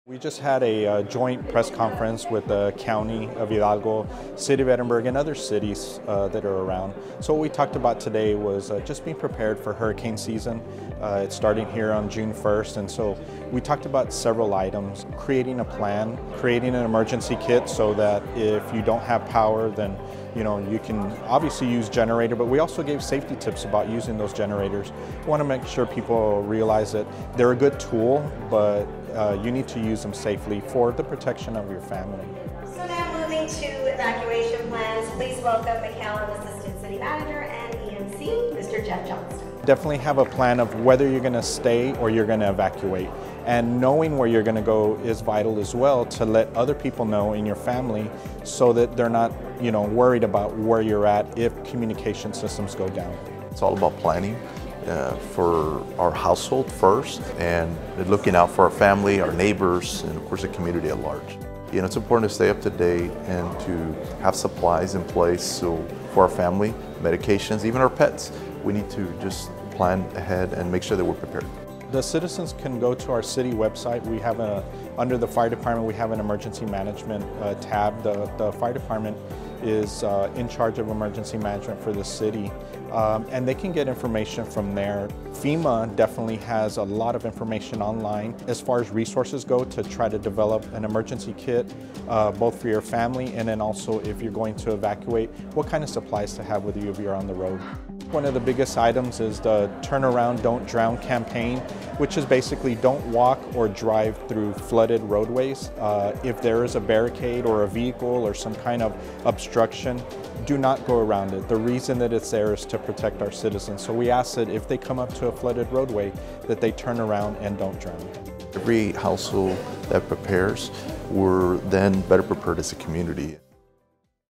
Hurricane Preparedness Press Conference 2023 Recap
🌪️🌊 Thanking our partners for a powerful collaboration! 🙌
We're incredibly grateful to our amazing neighboring cities, Hidalgo County and the Texas Division of Emergency Management , for joining forces with us in a joint regional hurricane preparedness press conference. Together, we're determined to keep our residents safe and ready for the upcoming hurricane season. 🏘️